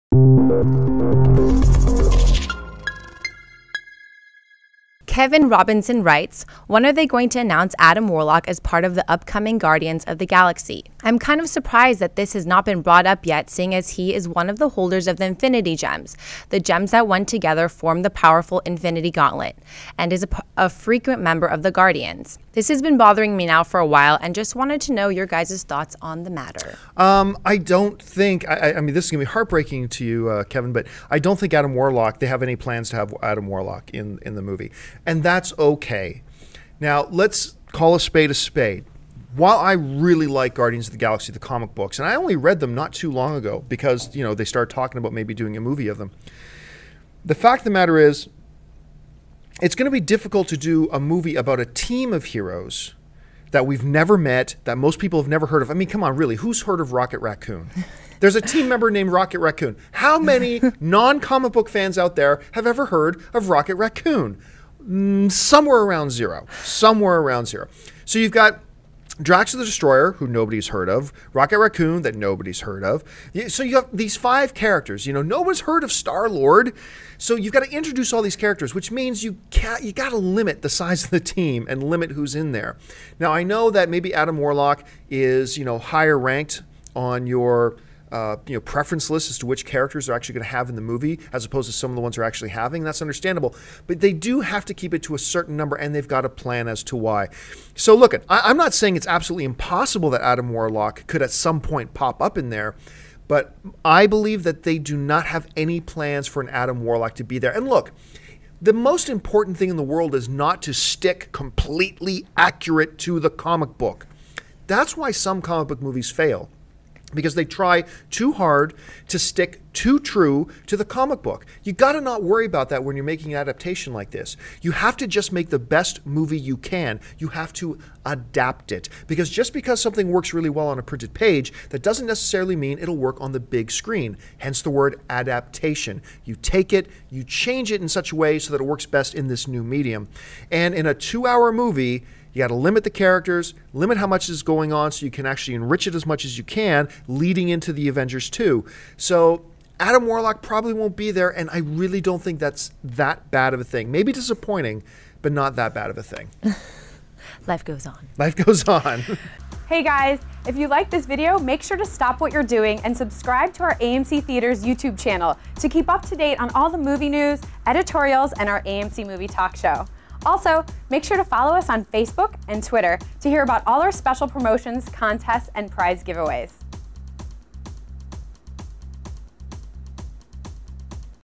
Adam Warlock in GUARDIANS OF THE GALAXY? - AMC Movie News
In a user-submitted question, we are asked if we think Adam Warlock will be announced to join the characters in Guardians of the Galaxy